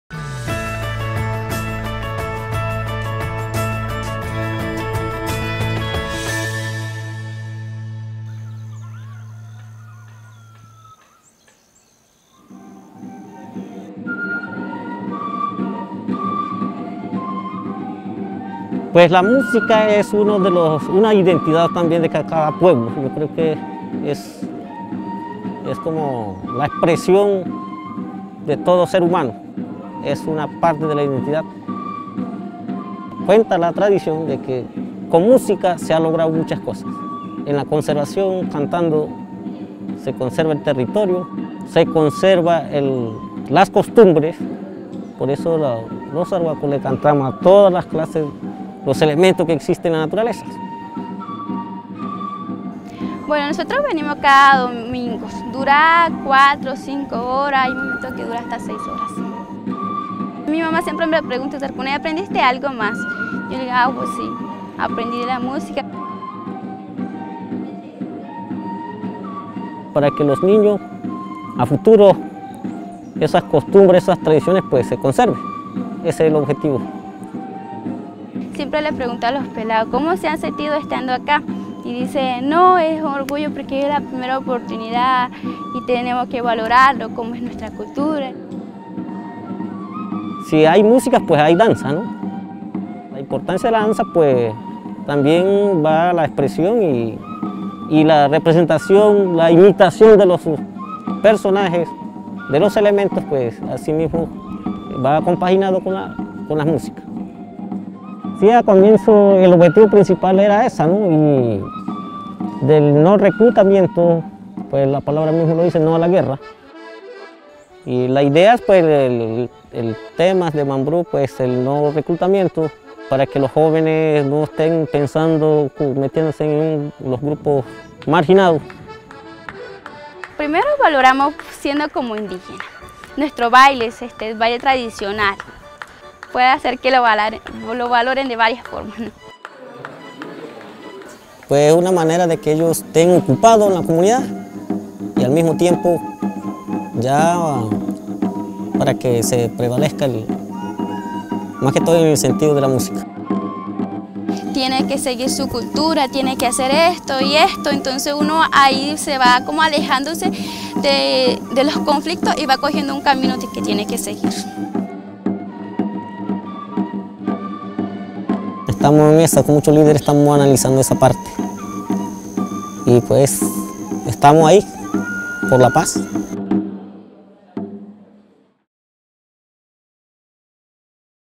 FrutosDePaz: La música y la danza ayudar a prevenir el reclutamiento
Niños y niñas indígenas arhuacos se benefician de la estrategia 'Mambrú No Va A La Guerra', liderada por la Agencia Colombiana para la Reintegración.